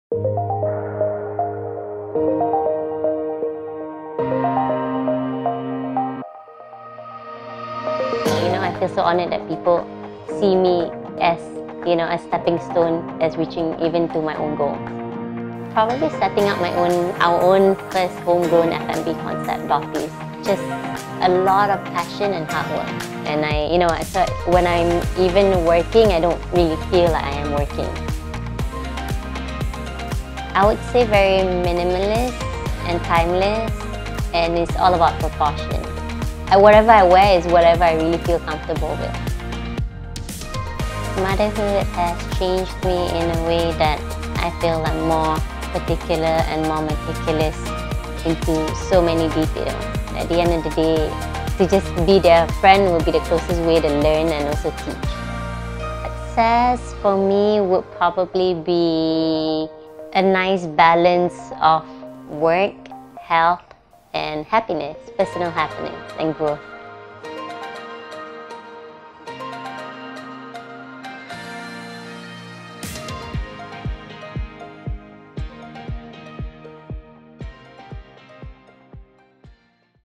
Nadia Nasimuddin is the Woman on the Rise | MALAYSIA TATLER
Malaysia Tatler’s 2017 Woman on the Rise shares an exclusive insight to her lifestyle philosophy while taking us through her preparation routine for social functions.